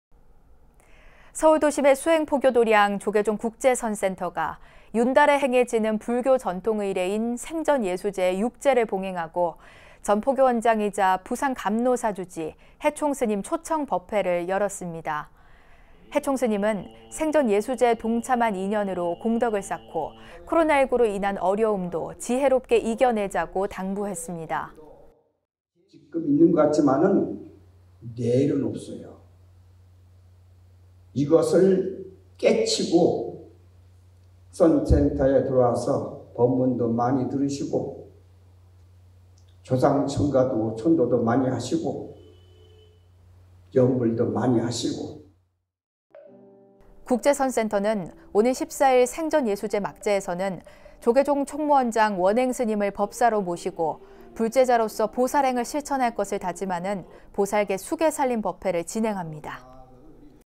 [BBS뉴스] 국제선센터 생전예수재 6재...혜총스님 초청 법회
서울 도심의 수행 포교 도량 조계종 국제선센터가 윤달에 행해지는 불교 전통의례인 생전예수재 6재를 봉행하고 전 포교원장이자 부산 감로사 주지 혜총스님 초청 법회를 열었습니다.

혜총스님은 생전예수재에 동참한 인연으로 공덕을 쌓고 코로나19로 인한 어려움도 지혜롭게 이겨나자고 당부했습니다.

[혜총스님/전 조계종 포교원장.부산 감로사 주지
지금 있는 것 같지만 내일은 없어요. 이것을 깨치고 선센터에 들어와서 법문도 많이 들으시고 조상 천도도 많이 하시고 염불도 많이 하시고...

국제선센터는 오는 14일 생전예수재 막재에서는 조계종 총무원장 원행 스님을 법사로 모시고 불제자로서 보살행을 실천할 것을 다짐하는 보살계 수계산림법회를 진행합니다